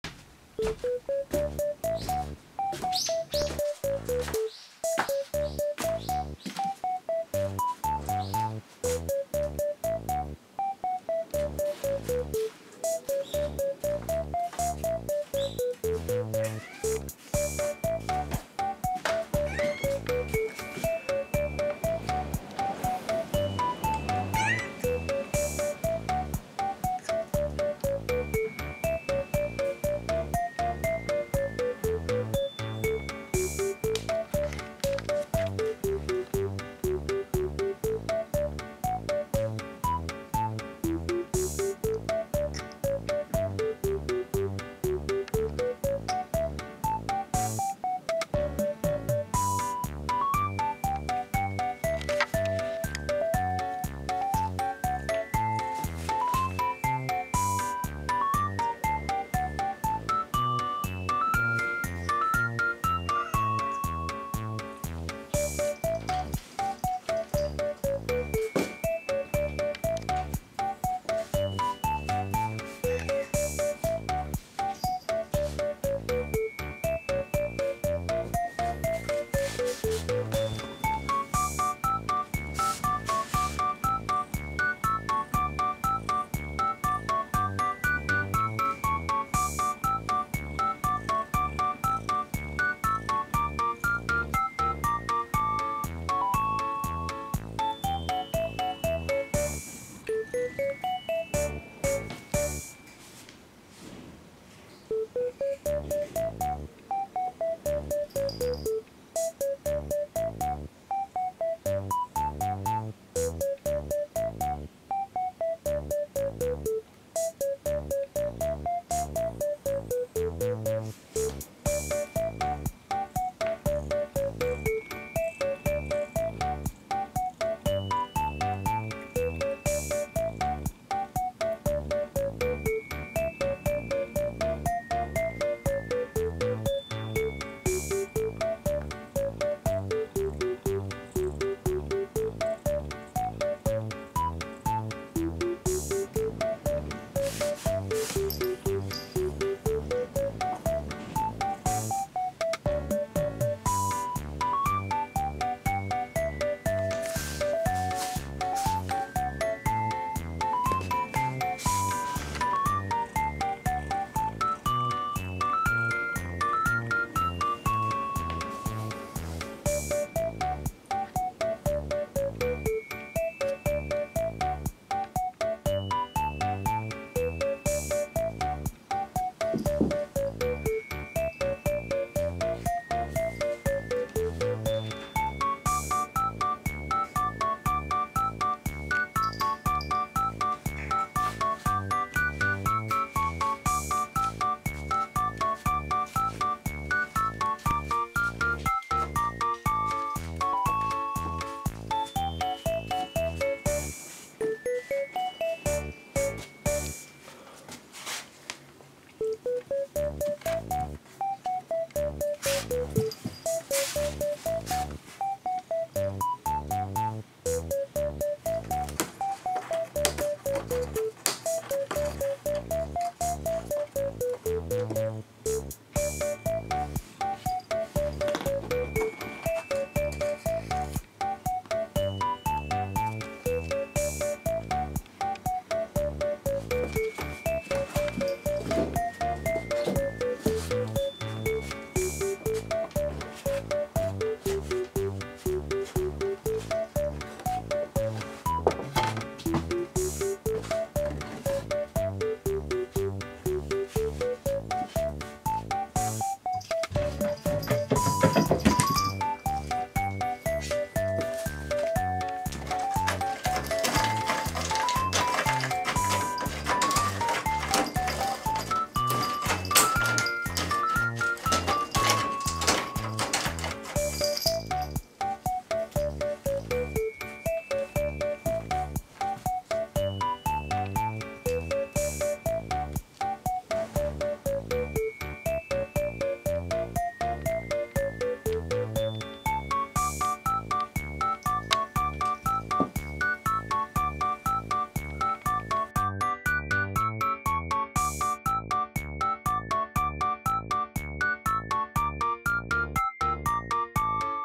あとは任せる。何かやってくれ猫さん【侵入した野良仔猫】～家猫修行中～
暑くてだるいわ！
何かとっておきのネタでも見せてくださいよ猫さん！